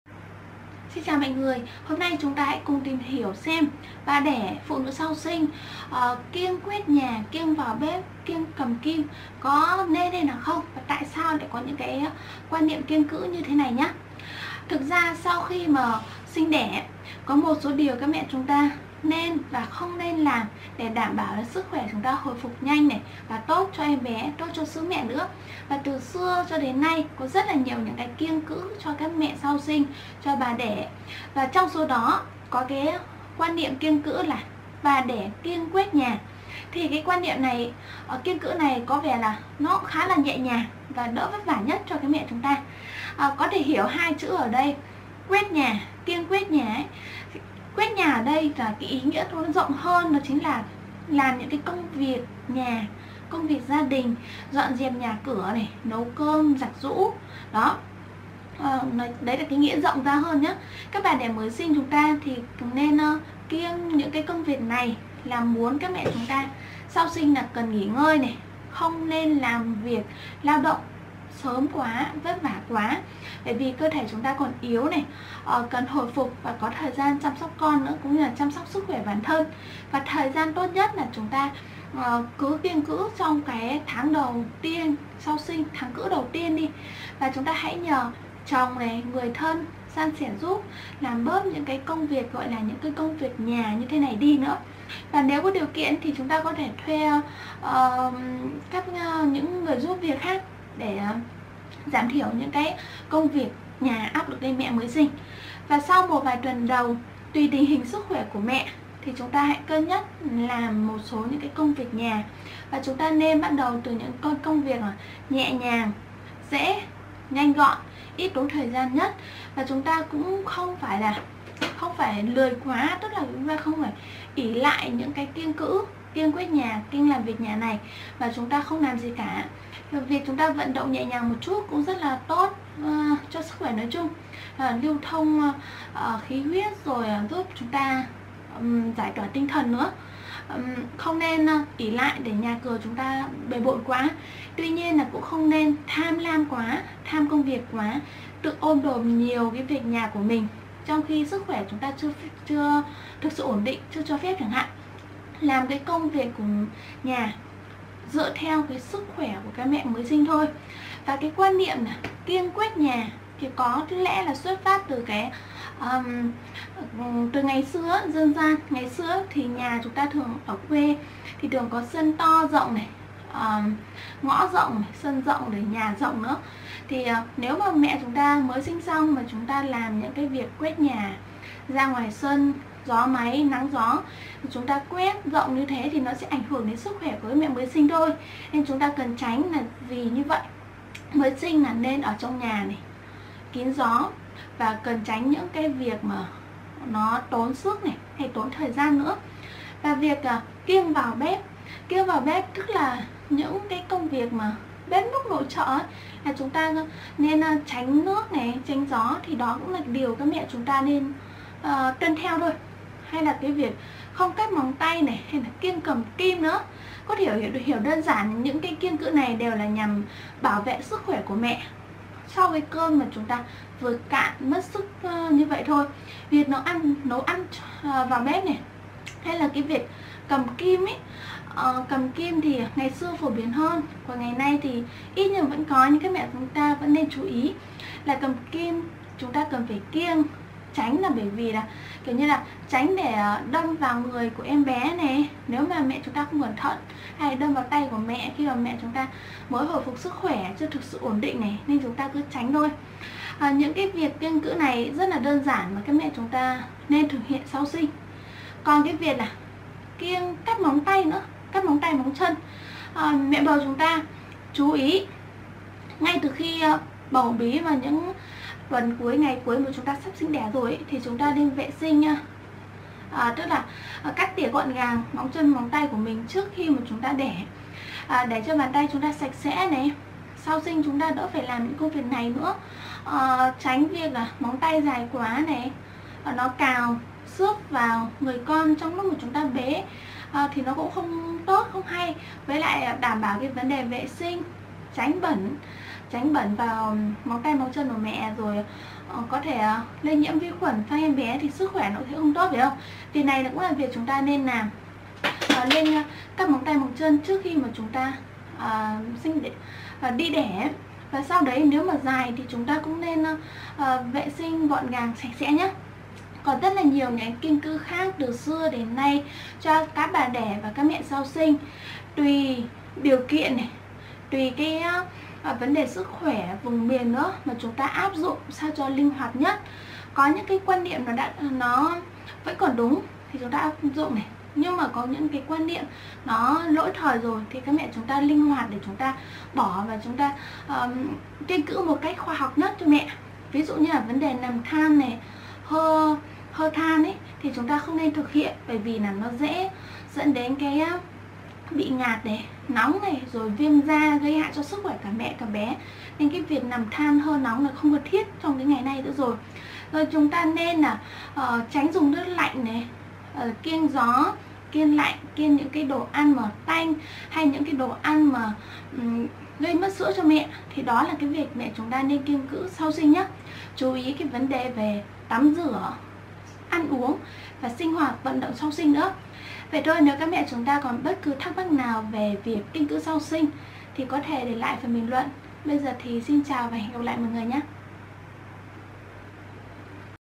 Tại sao bà đẻ phụ nữ sau sinh phải kiêng quét nhà, kiêng vào bếp, kiêng cầm kim?
Ngoài việc bà đẻ kiêng quét nhà, còn nhiều kiêng cữ theo dân gian được nhiều mẹ tin tưởng và áp dụng đến tận bây giờ. Như việc bà đẻ kiêng vào bếp, không được cắt móng tay, kiêng cầm kim...